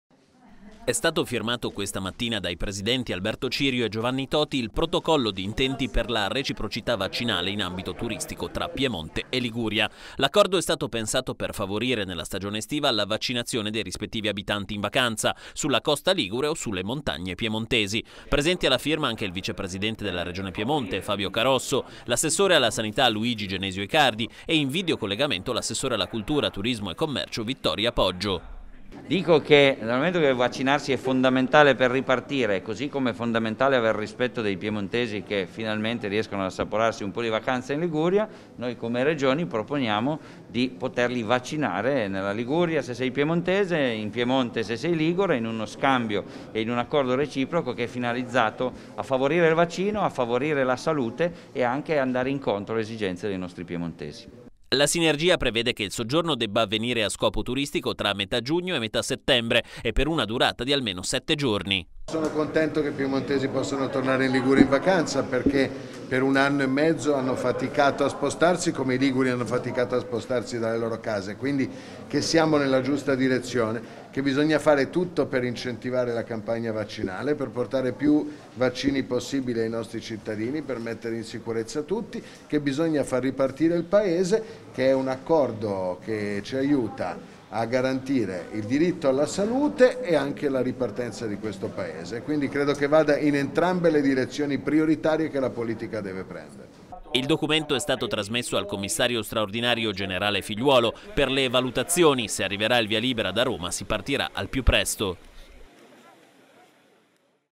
Accordo tra Piemonte e Liguria per vaccinare i vacanzieri
Sinergia tra Piemonte e Liguria per favorire nella stagione estiva la vaccinazione dei rispettivi abitanti in vacanza sulla costa ligure o sulle montagne piemontesi: è quanto prevede il protocollo d’intenti per la reciprocità vaccinale in ambito turistico firmato a Torino dai presidenti Alberto Cirio e Giovanni Toti. ll documento è stato già trasmesso al generale Figliuolo, commissario straordinario per l’Emergenza Covid-19, per ogni opportuna valutazione e se arriverà il via libera da Roma si partirà al più presto.